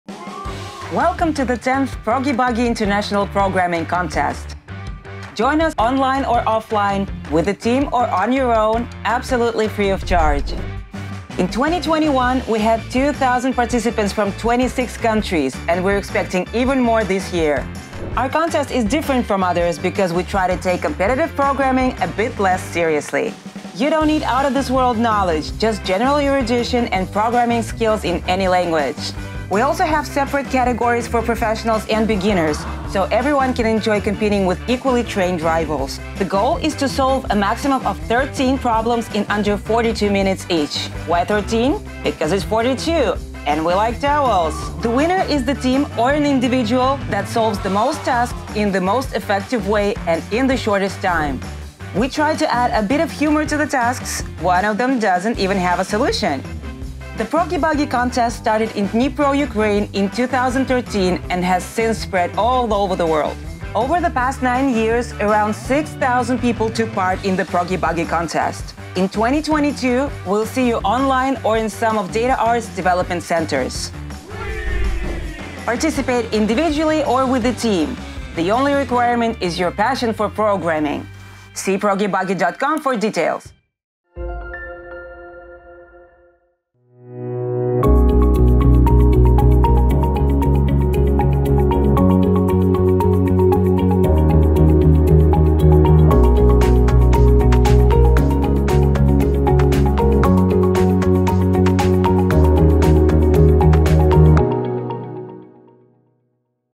The International Proggy-Buggy Contest, 2022, November, 12
Participation in the contest is free of charge.
🗓We start on November 12, 2022, at 12:00 (GMT).